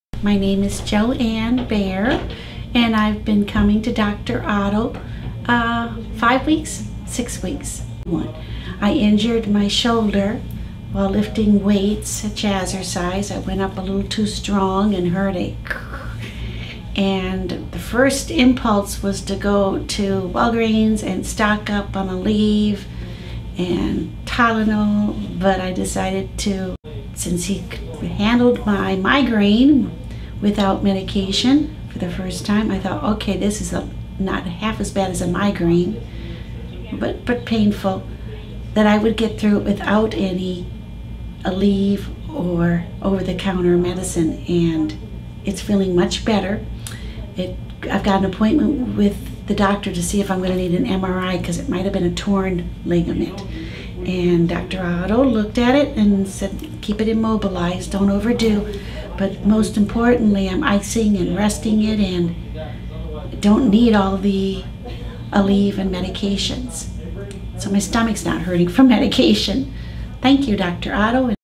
Jo Ann - Shoulder Injury helped at our orange park chiropractic clinic
Using a light gentle instrument, called an Impulse adjusting gun, we have shoulder protocols that test, confirm, lead to adjusting of the area affected, and great shoulder relief.  All cases respond differently, meaning come in to the office to get your shoulder evaluated by Dr. Otto, Orange Park Chiropractor